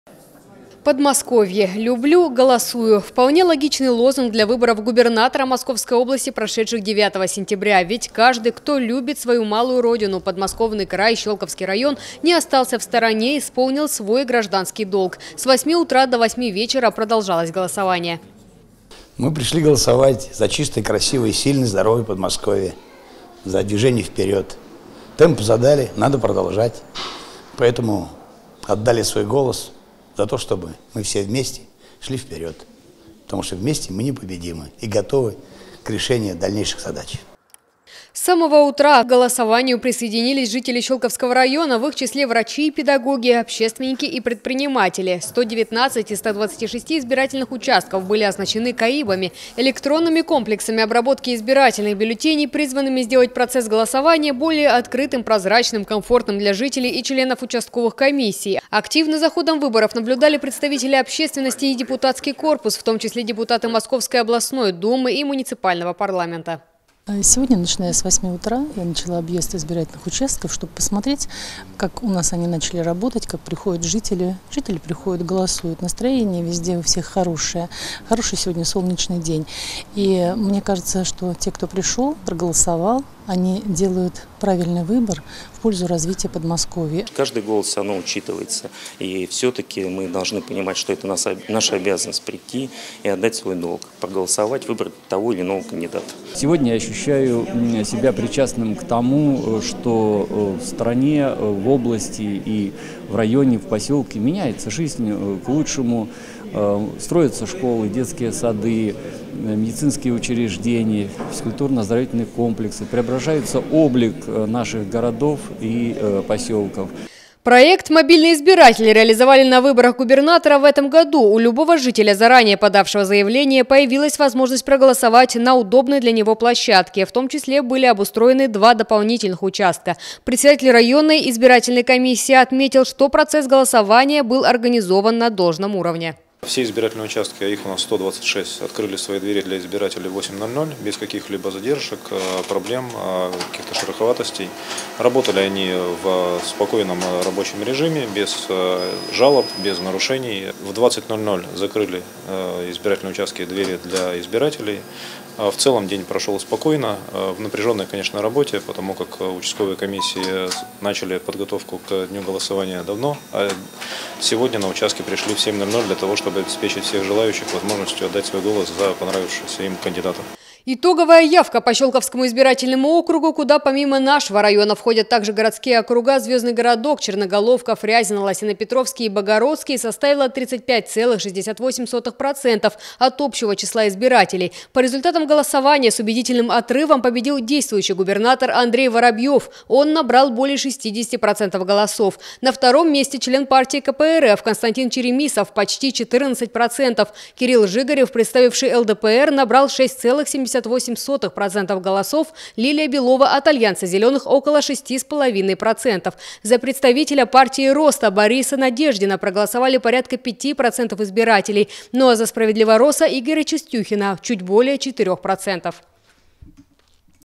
А.Воробьёв одержал победу на выборах Губернатора
Андрей Воробьёв победил на выборах губернатора Московской области. 9 сентября в Единый день голосования жители региона, в том числе и Щёлковского района, выбирали губернатора. За пост боролись 6 кандидатов. О процессе и результатах голосования в нашем муниципалитете, смотрите далее.